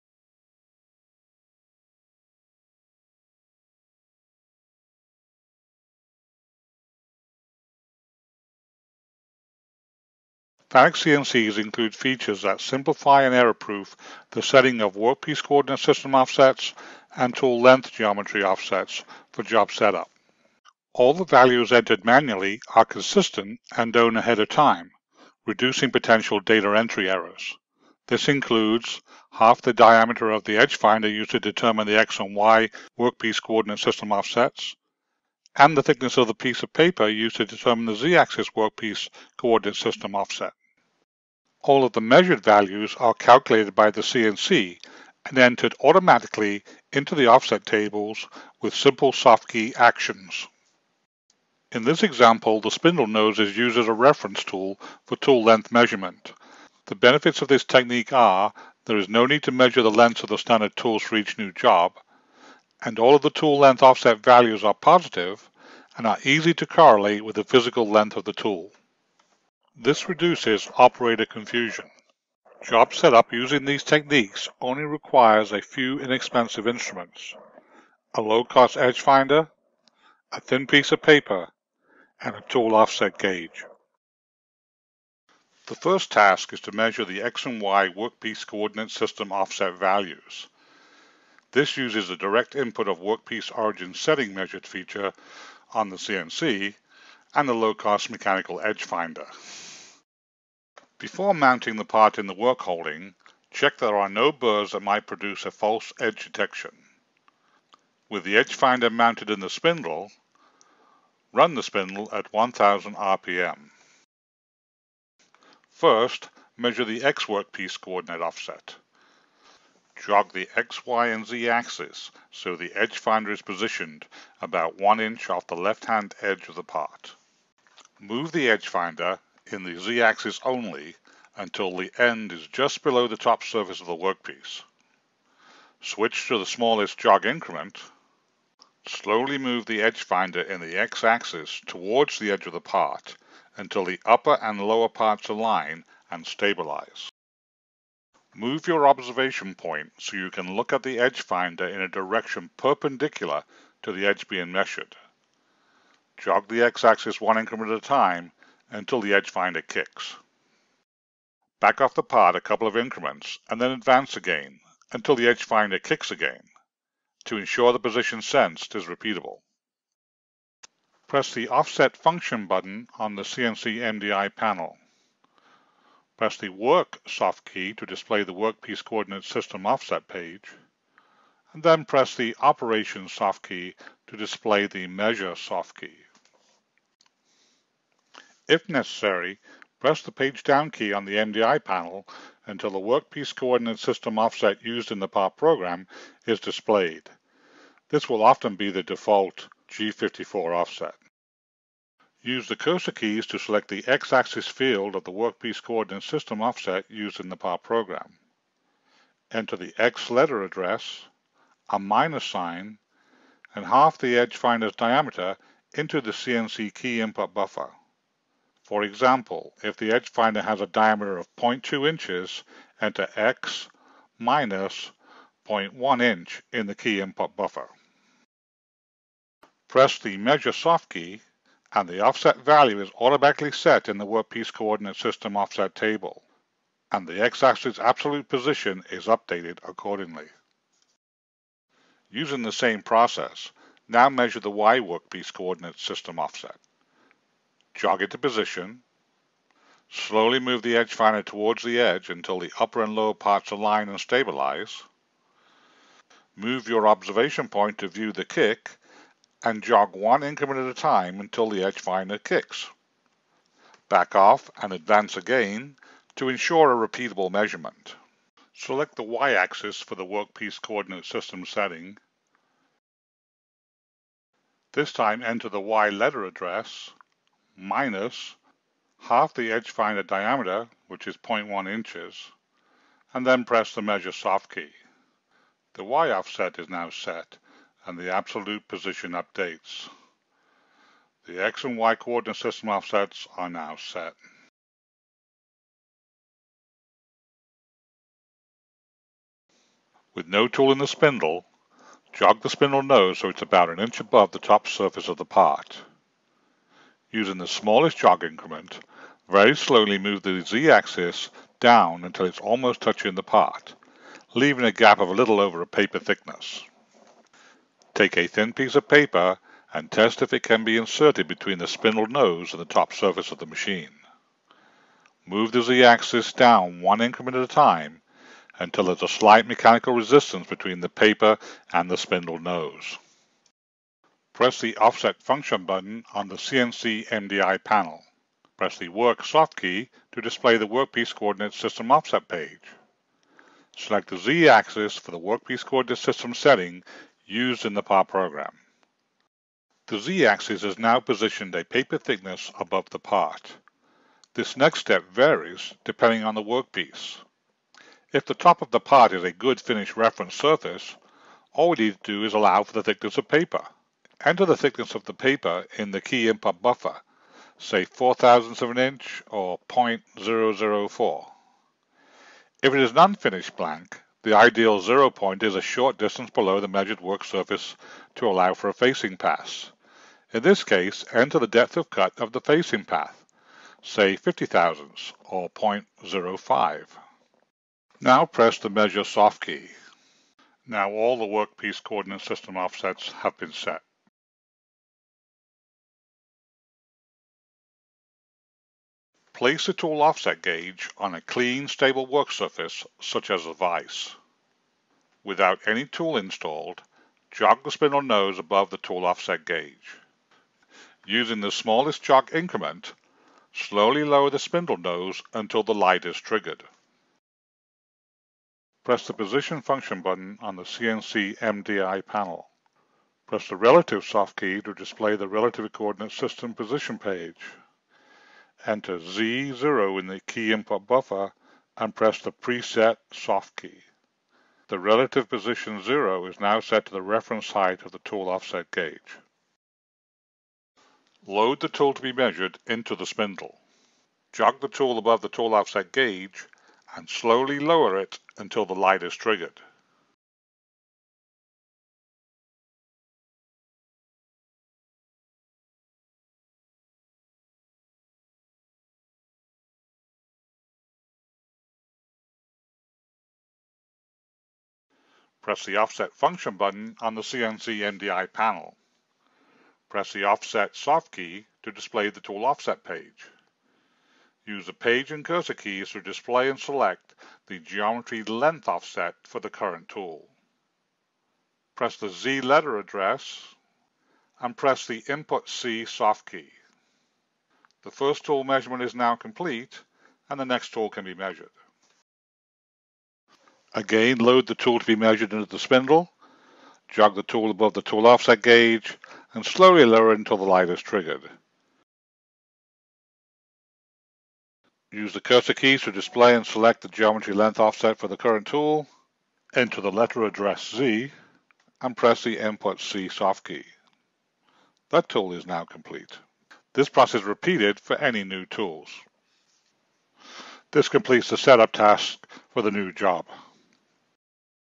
Easy Job Setup
FANUC controls include features to simplify job setup. This video demonstrates workpiece coordinate offset measurement and tool length measurement using the CNC and a few low cost tools and with the minimum data entry.